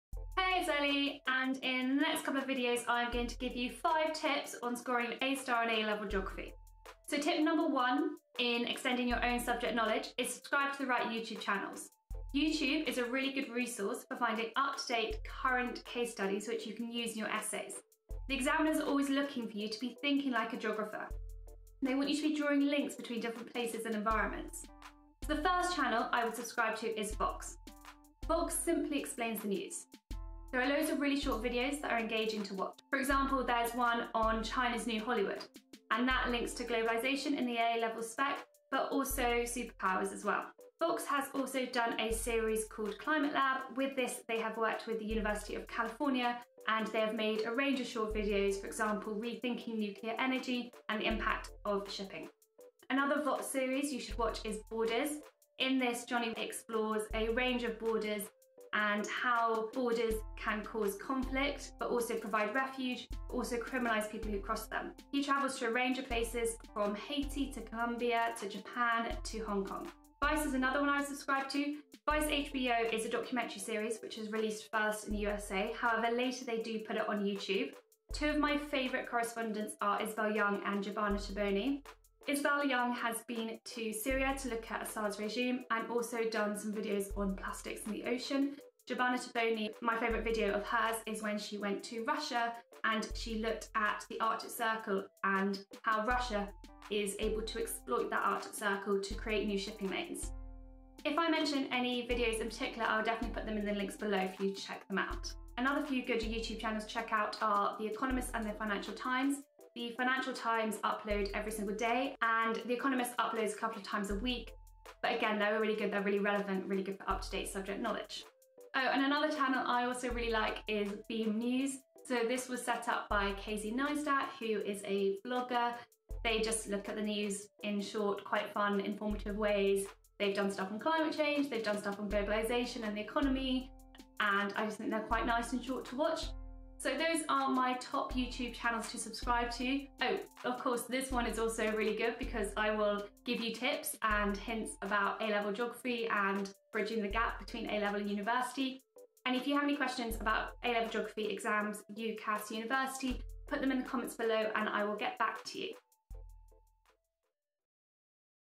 Geography Channels I A*STAR in A Level Geography TIP #1/5 I AQA Edexcel OCR
These YouTube channels will extend your subject knowledge to help you get the top grades, enabling you to wow the examiners.

To get an A star the examiners want to see you are thinking like a geographer and interested in the world around you - subscribing to the following YouTube channels will increase your knowledge and synoptic understanding.